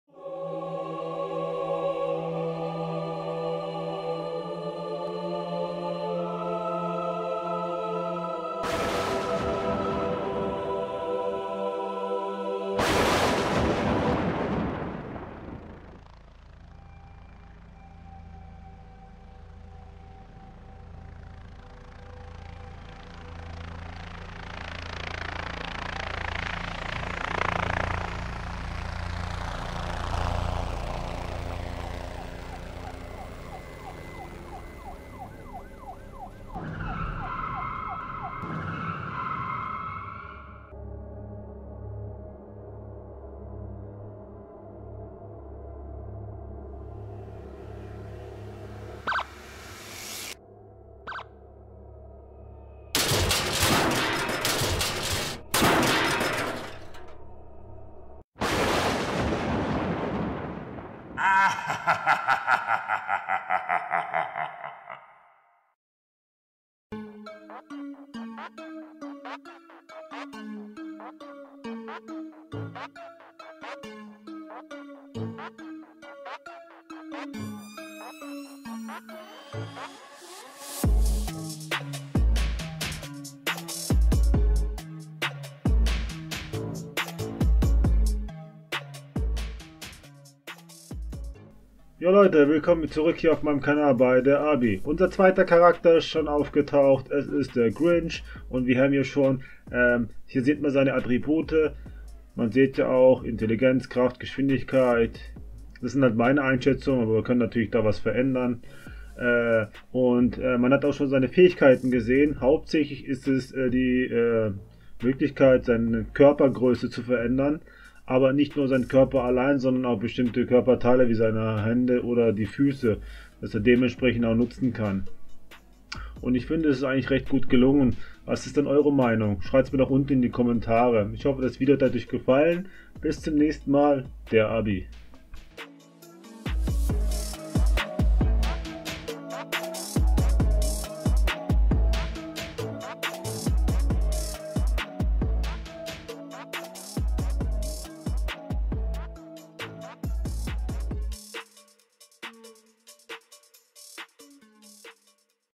Dieser Charakter wird nur Ärger machen ! #3
Unser neuer Charakter hat eine ganz andere Einstellung als der Osterhase!, vielleicht kann er ja doch noch hilfreich sein ?

Meine Programme, alles Open Source!

Social Media